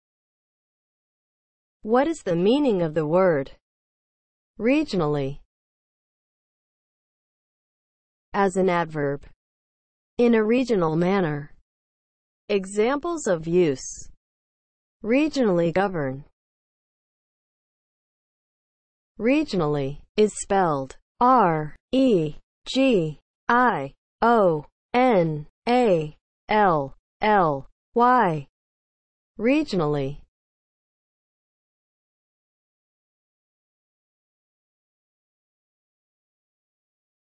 What is the meaning of the word REGIONALLY?
Definition, examples of use and spelling of the word REGIONALLY.

(00:00) Intro
(00:07) As an adverb
(00:17) Spelling

As an adverb (00:07):
• in a regional manner

Examples:
• "regionally governed"